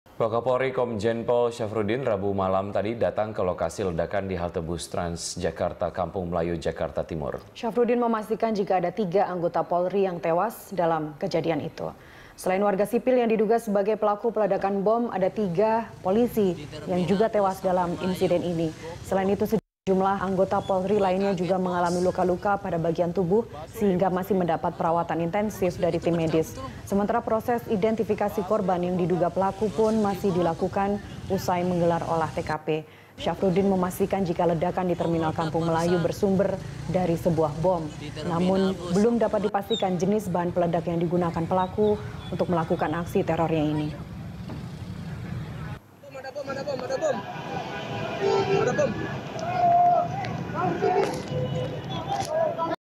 Wakapolri: 3 Anggota Polri Tewas dalam Ledakan di Kampung Melayu
Wakapolri Komjen Pol Syafruddin datang ke lokasi ledakan bom yang terjadi di Kampung Melayu, Jakarta Timur, Rabu (24/05/2017) malam. Syafruddin memastikan tiga anggota Polri tewas dalam ledakan tersebut.

Facebook.com/BeritaSatuTV
Youtube.com/BeritaSatu